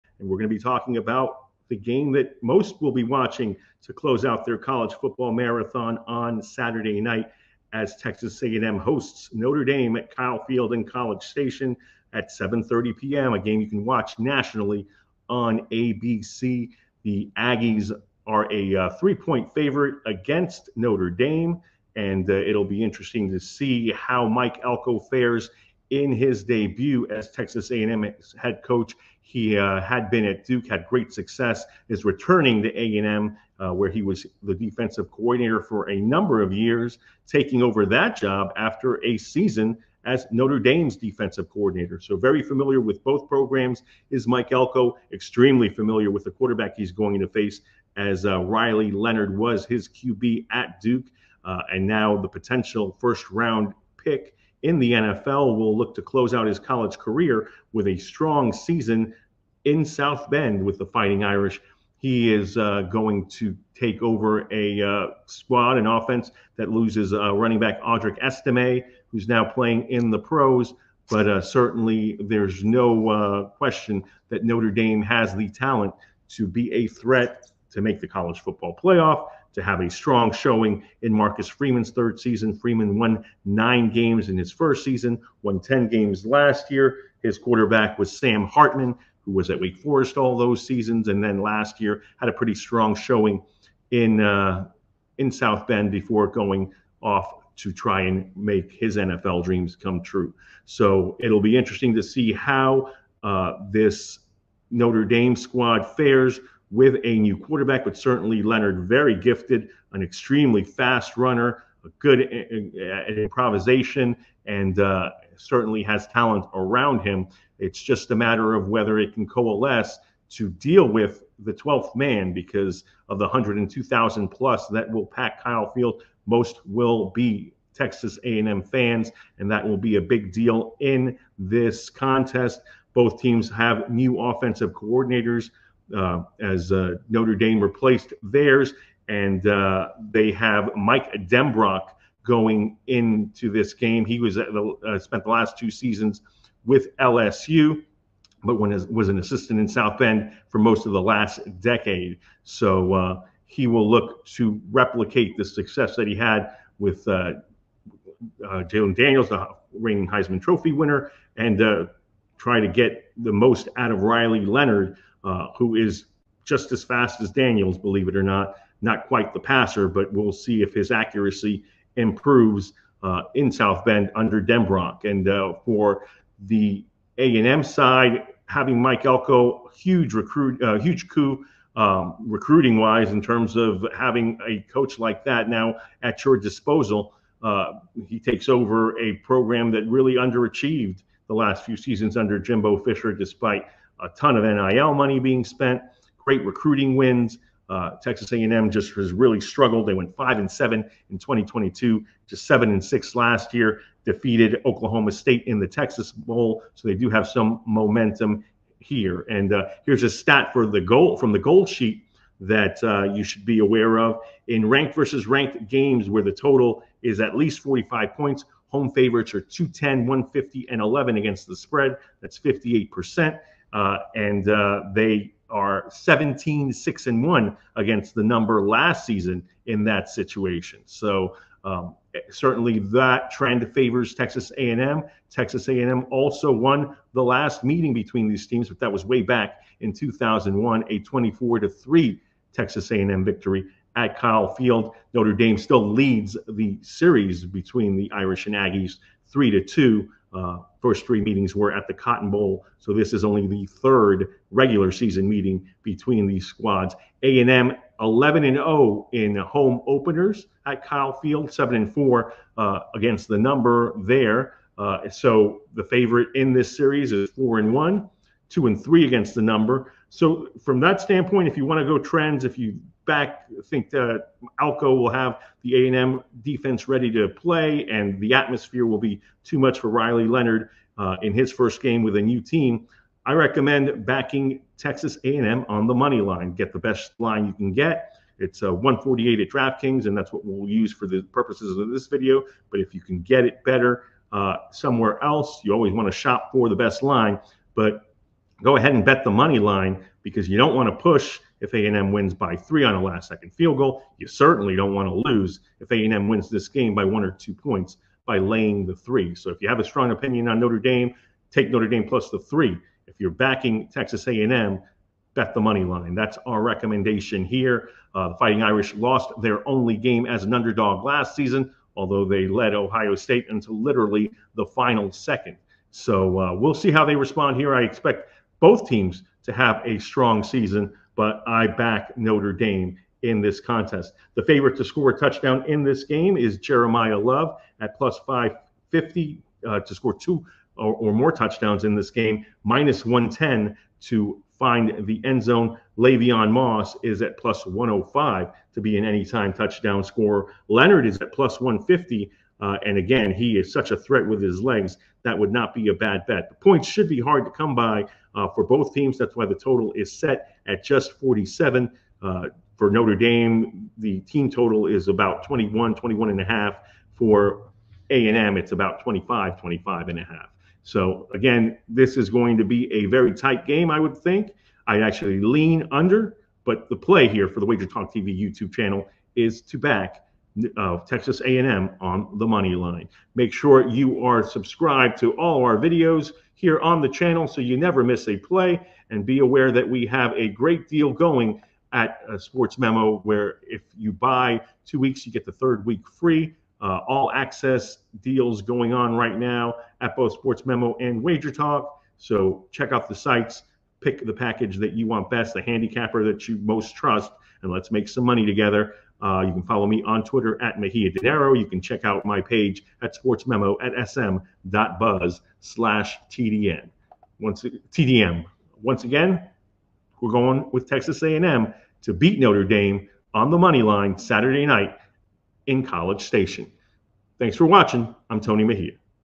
Notre Dame vs Texas A&M Prediction, Picks and Best Bets | 2024 College Football Picks Week 1
If you are looking for a College Football Week 1 betting preview with picks and predictions for the Notre Dame vs Texas A&M matchup, we have you covered! Our college football handicapping experts discuss this matchup from a sports betting perspective, sharing their picks and predictions vs. the current odds.

Notre Dame Fighting Irish vs Texas A&M Aggies: A Clash of College Football Titans

The college football season is kicking off with a bang as the Notre Dame Fighting Irish (@NDFootball) face off against the Texas A&M Aggies (@AggieFootball) on August 31, 2024. This highly anticipated matchup will take place at the iconic Kyle Field in College Station, Texas.

Location: Kyle Field in College Station, Texas
Kickoff Time: 7:30 PM ET / 4:30 PM PT
How to Watch: ABC

Betting Info:
Current Spread: Texas A&M -3

Interesting Facts:
Notre Dame Fighting Irish: The Fighting Irish are known for their storied history and national prominence. They'll be looking to start their season strong with a win on the road.

Texas A&M Aggies: The Aggies are playing at home in front of their passionate 12th Man. They'll be eager to defend their home turf and make a statement against a top-tier opponent.

Key Storylines:
- New Faces: Both teams will feature new players and coaches, adding an element of unpredictability to the game.
- Strength vs. Strength: Notre Dame's offense will be tested against Texas A&M's stout defense.
- Road Warriors: The Fighting Irish will need to overcome the challenges of playing in a hostile environment.

This matchup promises to be an exciting clash of two powerhouse programs. Make sure to tune in and witness the action unfold!